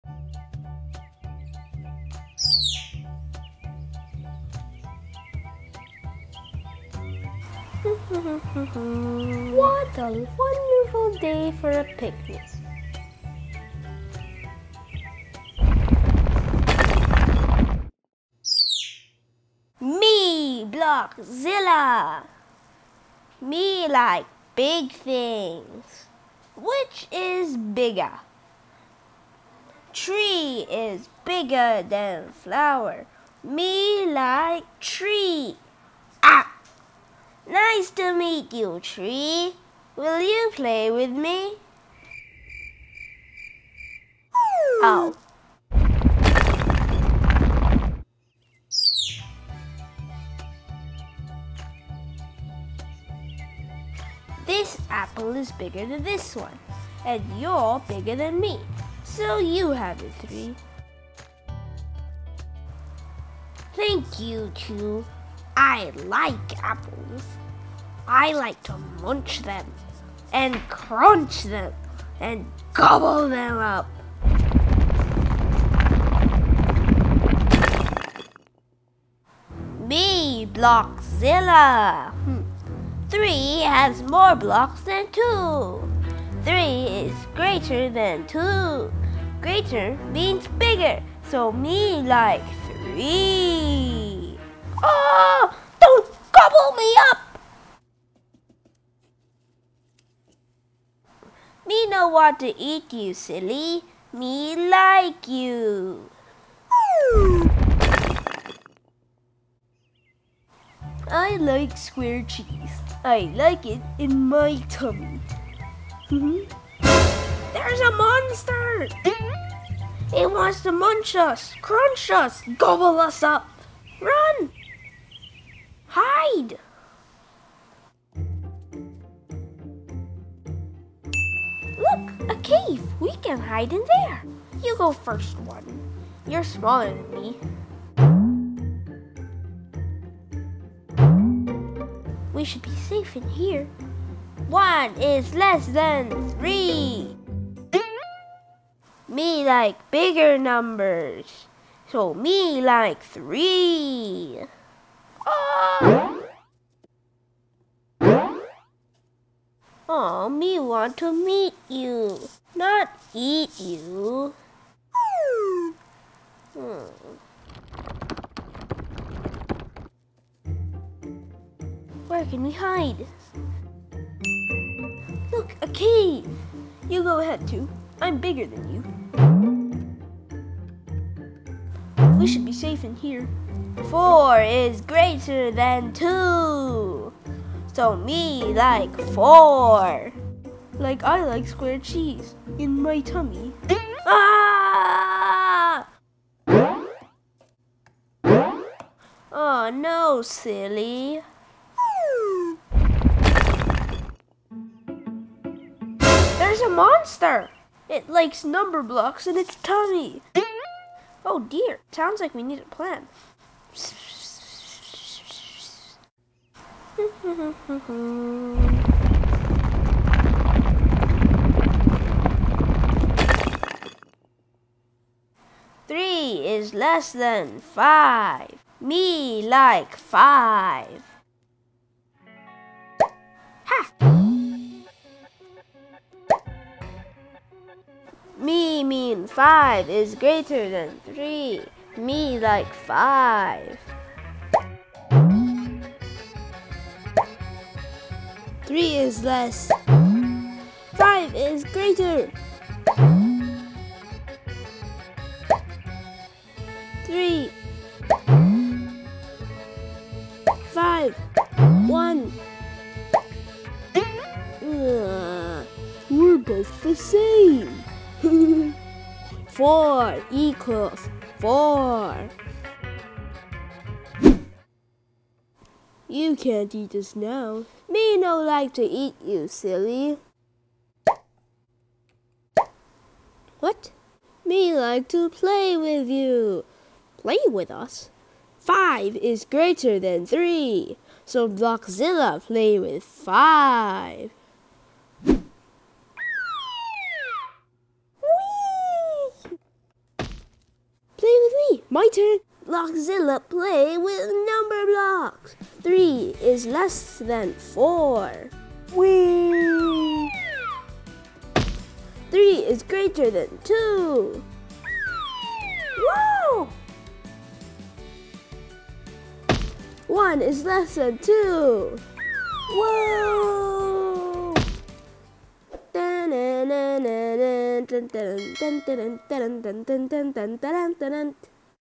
Blockzilla Meets Numberblocks (Learn to Compare Numbers) || Keith's Toy Box
Blockzilla arrives in Numberland!  Kids can learn to compare numbers from this episode.  Blockzilla loves to play with bigger numbers, help him decide with numbers are greater than, less than, or equal!